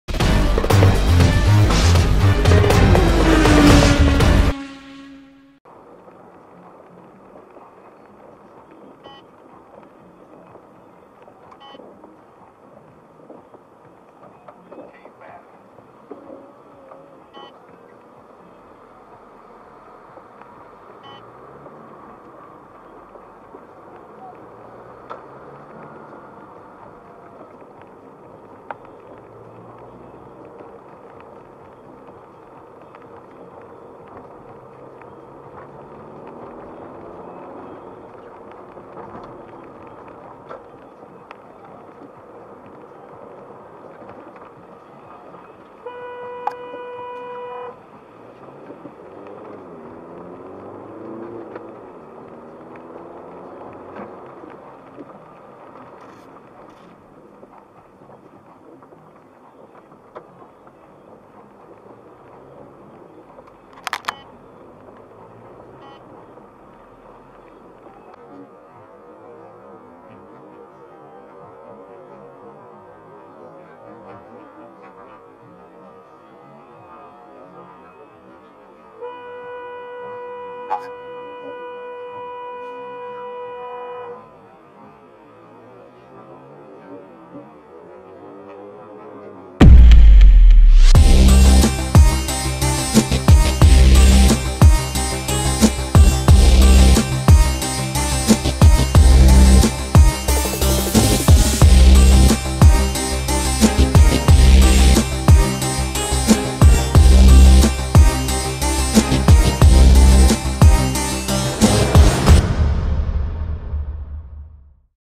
Un criminal in libertate - Trafic Botosani S01E29
Seara, ploaie, vizibilitate redusă, carosabil alunecos etc.
No comment!
Un viitor criminal în libertate pe străzile Botoşaniului.
Spun asta pentru că la un moment dat va da peste un şofer mai neexperimentat sau neatent şi tragedia va fi inevitabilă.